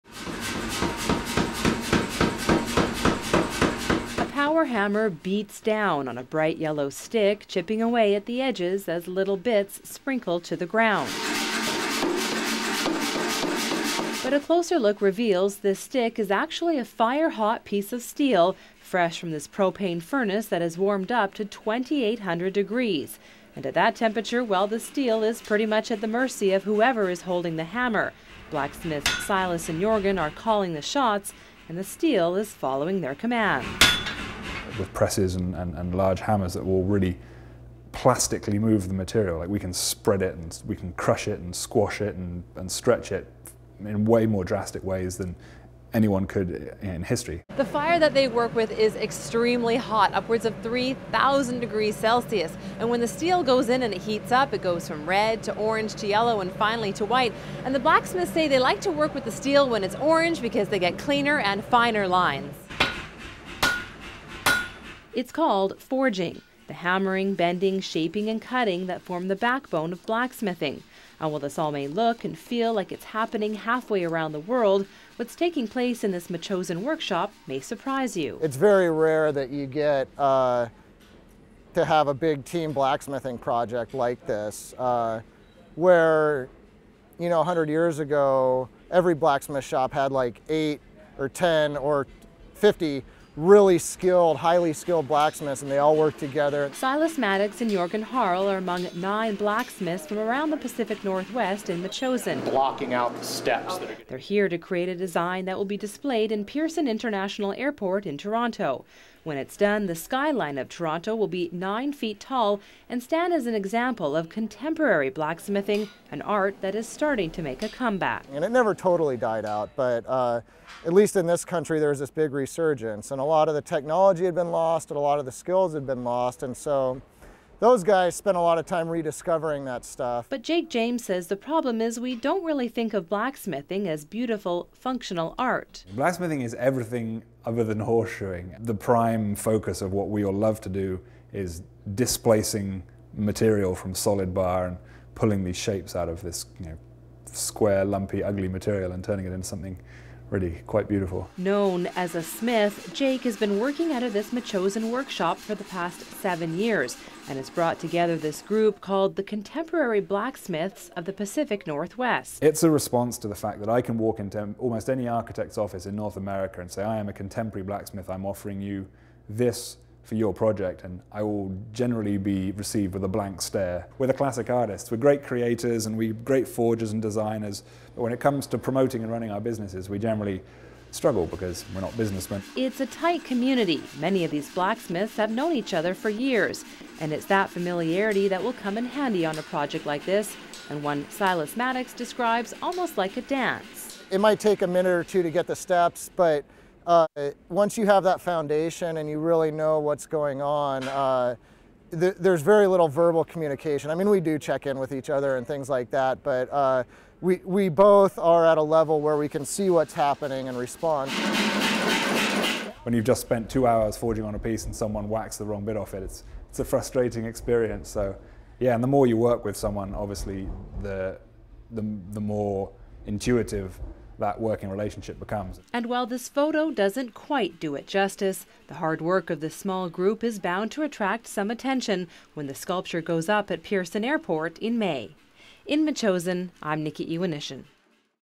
Blacksmith Art in Metchosin - Shaw TV Victoria
Shaw TV's Nikki Ewanyshyn meets some blacksmiths in Metchosin who have taken their craft and turned it into art. Now the art will be the one of the first things seen when people come through Pearson International Airport in Toronto.